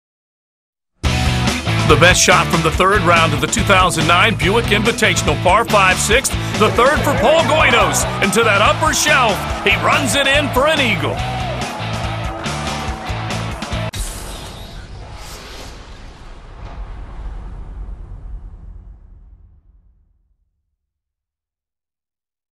Paul Goydos Chips In for Eagle: Shot of the Day
Paul Goydos chips-in for eagle on the par-5 6th hole at Torrey Pines during the third round of the 2009 Buick Invitational. See more PGA TOUR Shots of the Day